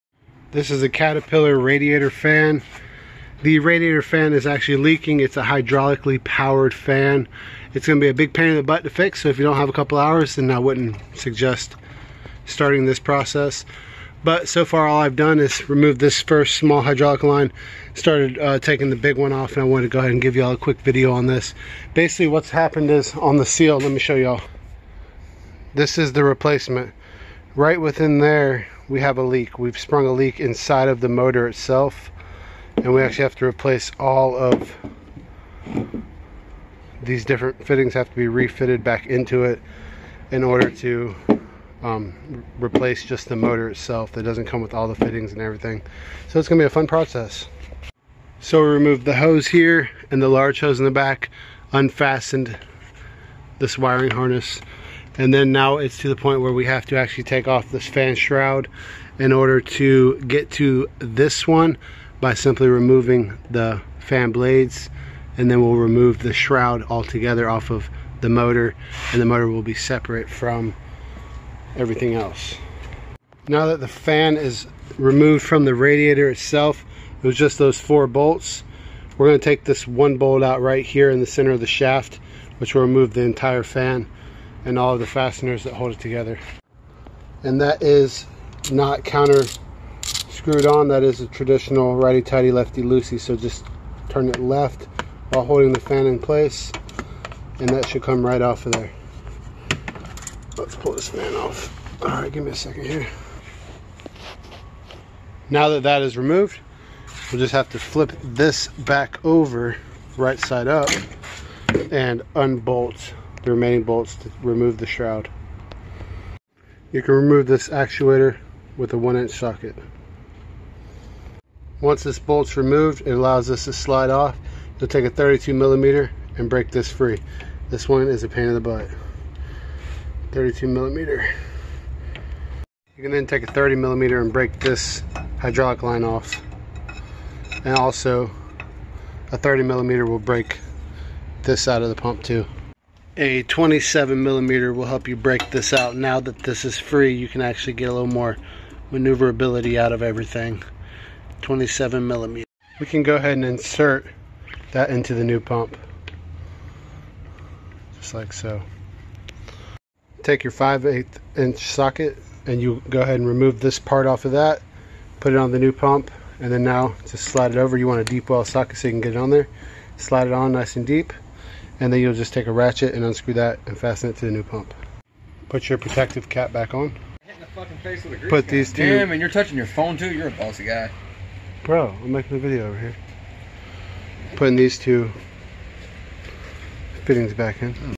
Cat skid steer hydrolic fan replacement.
oil leaking from Caterpillar radiator fan. How to replace the radiator fan hydraulic pump on a caterpillar skid steer. Hydraulic fan pump replacement. Caterpillar skid steer fan replacement